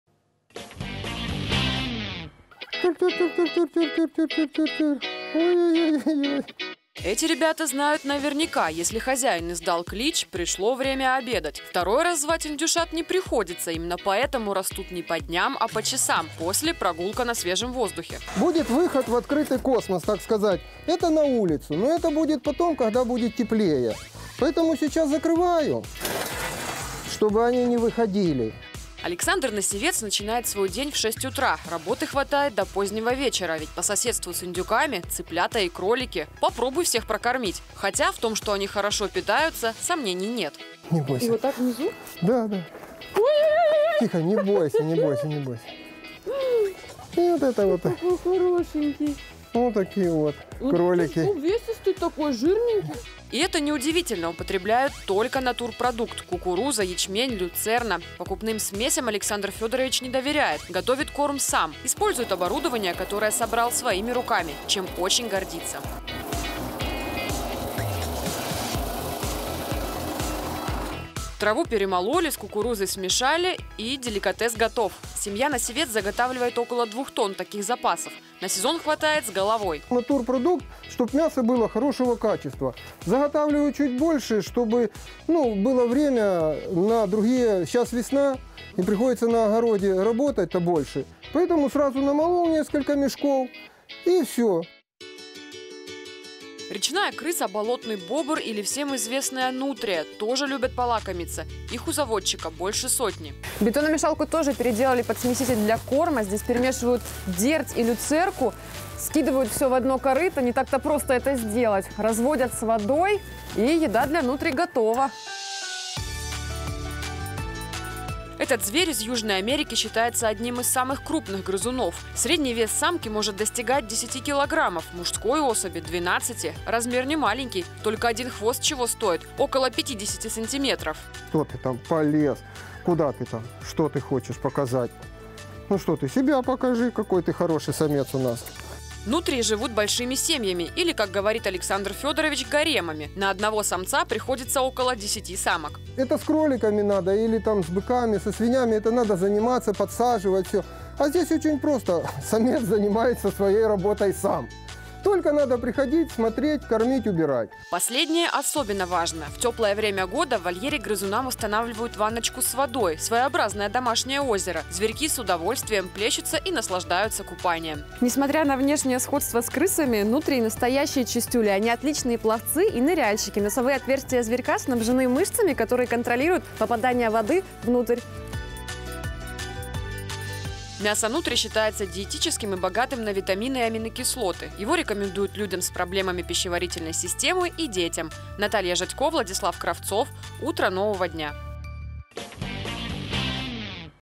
Коктейль из кукурузы, люцерны и дерти: сколько еды нужно домашним индюкам и нутриям?
Александр Носивец начинает свой день в шесть утра. Работы хватает до позднего вечера, ведь по соседству с индюками – цыплята и кролики. Попробуй всех прокормить. Хотя, в том, что они хорошо питаются – сомнений нет…